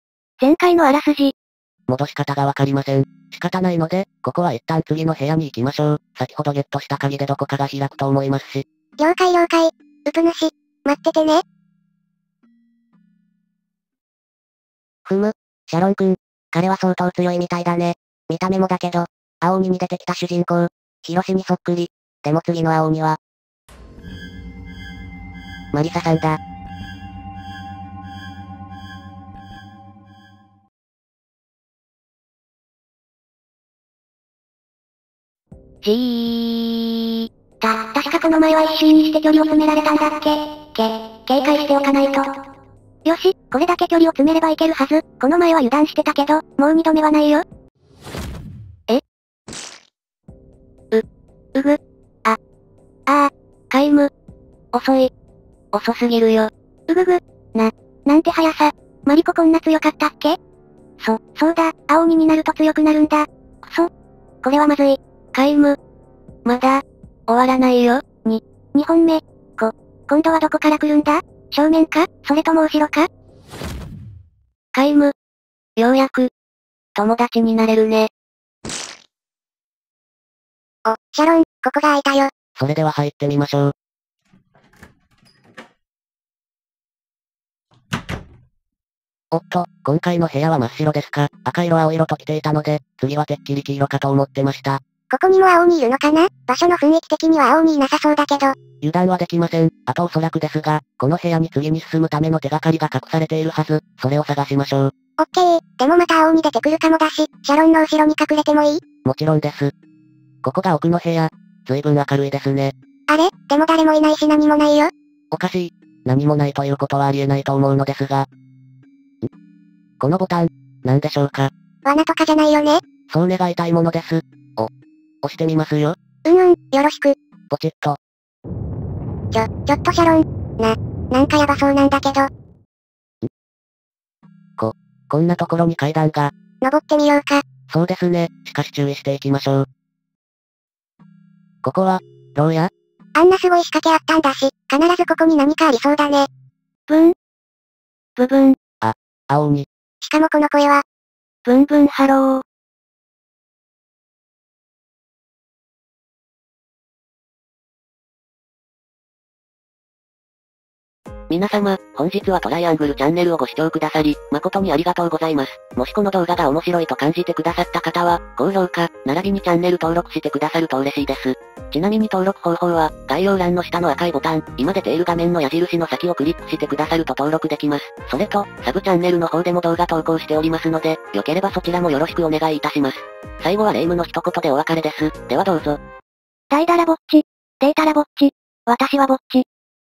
【ゆっくり茶番/第33話】青鬼が妹を食べる！！ 恐怖、青鬼の館編
今回はなんとまさかまさかのヒカキンさんが登場！？
しかも霊夢の姿をして！？
もちろんHikakin青鬼さんですが、その恐ろしい能力とは！？
恐怖と心霊現象満載のゆっくり青鬼ホラー茶番、是非お楽しみください＾＾